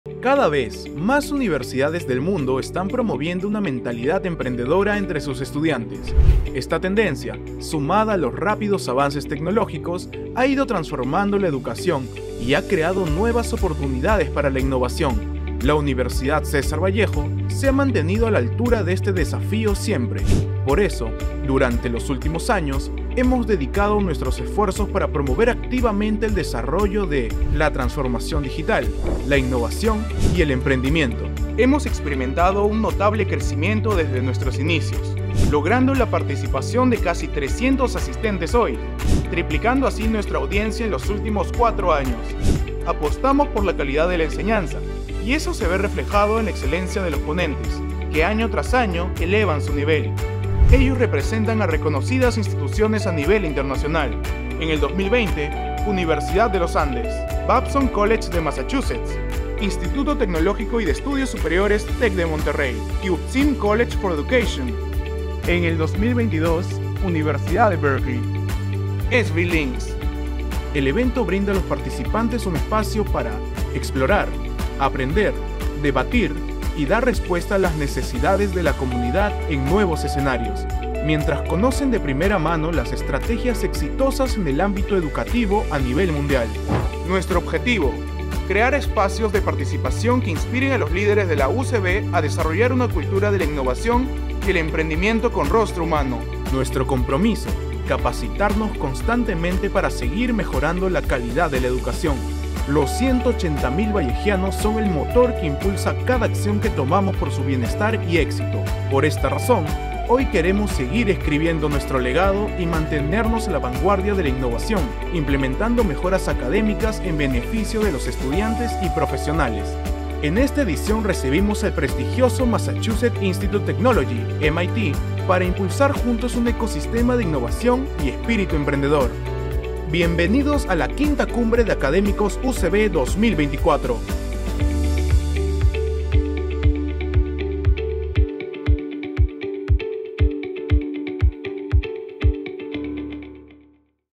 ¡Nada nos detiene! Cada año buscamos la mejora continua a través de capacitaciones internacionales.
Conoce las destacadas instituciones con las que nuestro equipo de representantes académicos se han reunido, año a año, para la mejora continua en el enfoque educativo y para seguir brindando la educación que nuestros vallejianos esperan.